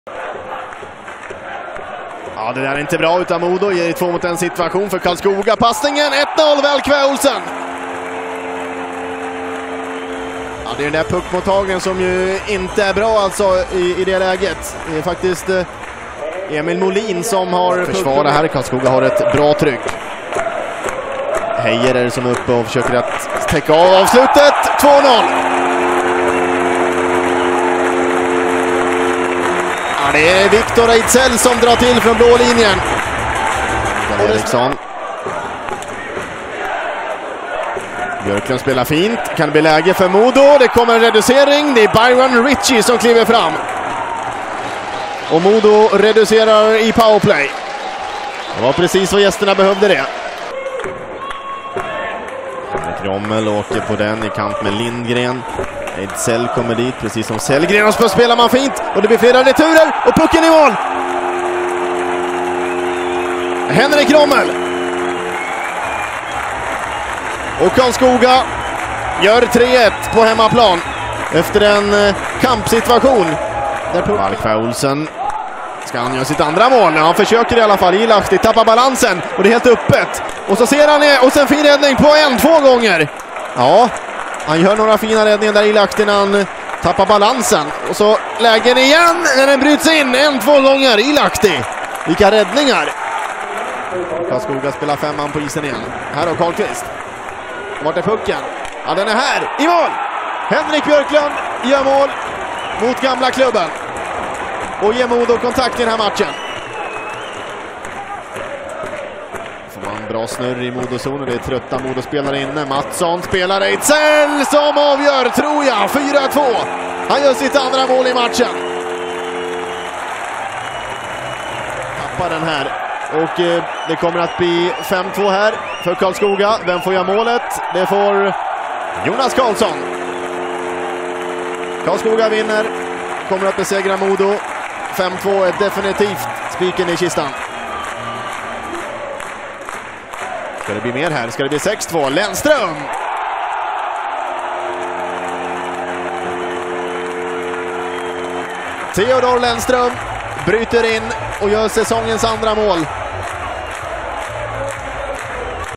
Höjdpunkter: Karlskoga hemmaslog Modo - TV4 Sport
TV4 Sport från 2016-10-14: Karlskoga hemmaslog Modo i Hockeyallsvenskan.